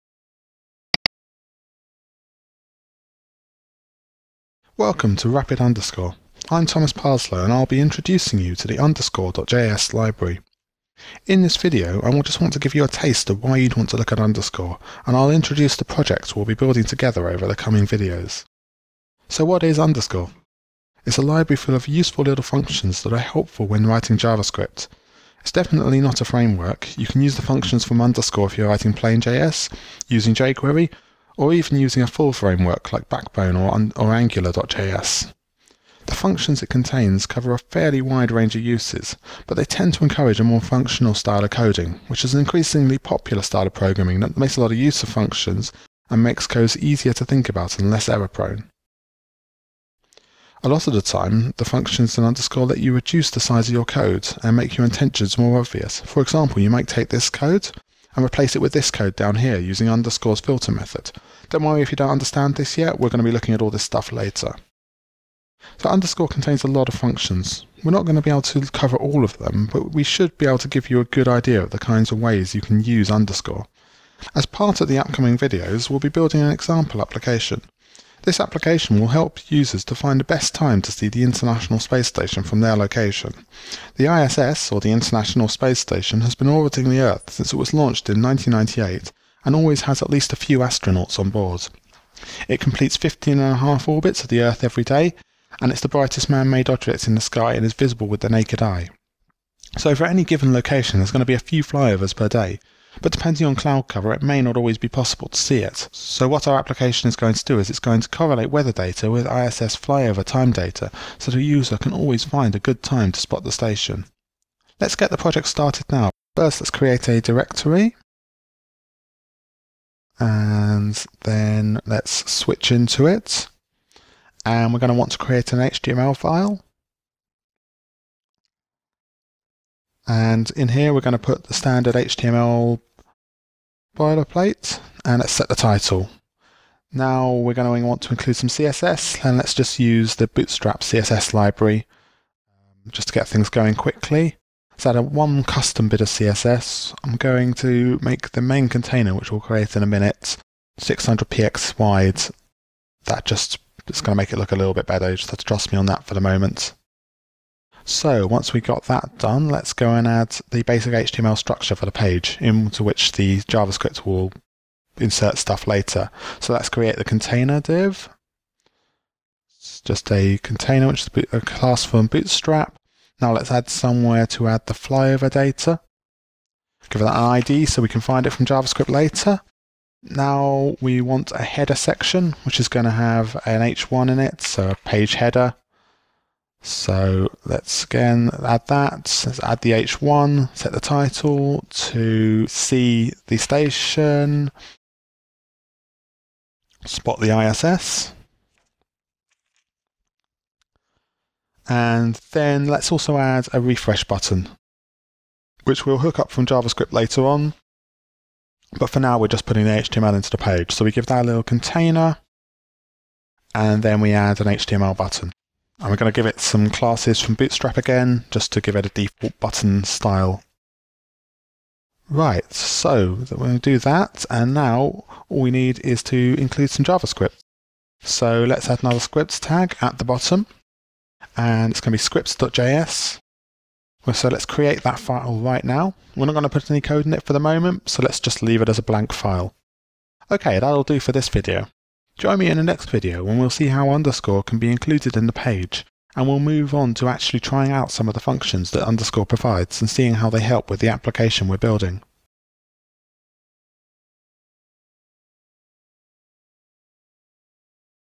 Rapid Underscore.js Tutorial: What Is Underscore.js? | packtpub.com
Discover why you might want to learn about Underscore.js.
o Take a quick tour of the Underscore library
o Get a glipmse of the example application